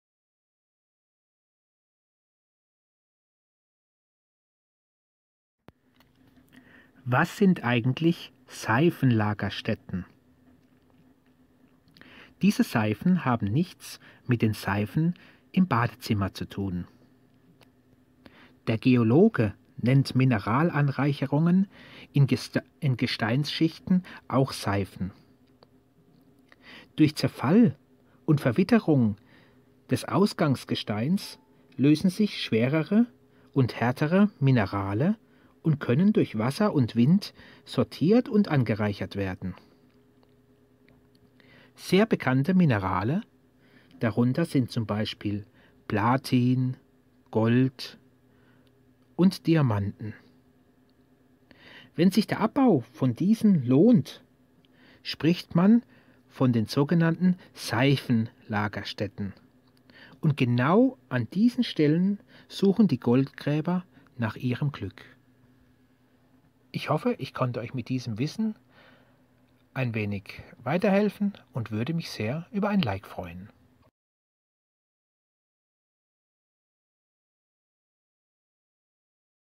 Was sind Seifenlagerstätten? Geschichte Herkunft Gestein Gesteinsschichten Gold Platin Diamanten
Was sind Seifenlagerstätten?
Diese Seifen haben nichts mit den Seifen in Badezimmern zu tun.
Der Geologe nennt Mineralanreicherungen in Gesteinschichten auch Seifen
Durch zerfall und Verwitterung des Ausgangsgesteins lösen sich schwerere und härtere Minerale und können durch Wasser und Wind sortiert und angereichert werden
Sehr bekannte Minerale darunter sind z.B. Platin, Gold und Diamanten
Wenn sich der Abbau von diesen lohn, sprich man von Seifenlagerstätten
Und genau an diesen Stellen suchen die Goldgräber nach Ihrem Glück